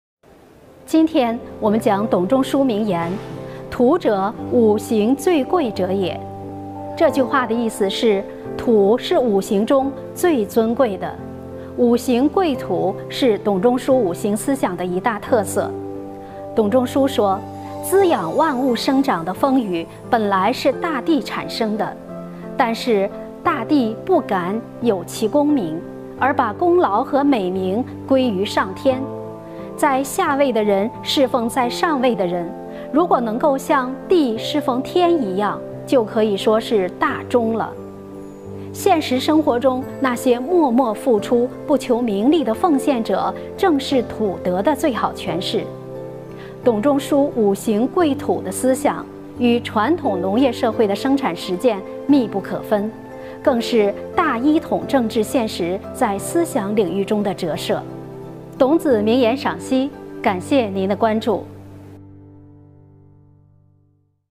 土者，五行最贵者也
今天我们讲董仲舒名言“土者，五行最贵者也”。这句话的意思是：土是五行中最尊贵的。“五行贵土”是董仲舒五行思想的一大特色。董仲舒说，滋养万物生长的风雨本来是大地产生的，但是大地“不敢有其功名”，而把功劳和美名归于上天。在下位的人事奉在上位的人，如果能够像地事奉天一样，就可以说是“大忠”了。现实生活中那些默默付出、不求名利的奉献者正是土德的最好诠释。董仲舒“五行贵土”的思想，与传统农业社会的生产实践密不可分，更是大一统政治现实在思想领域中的折射。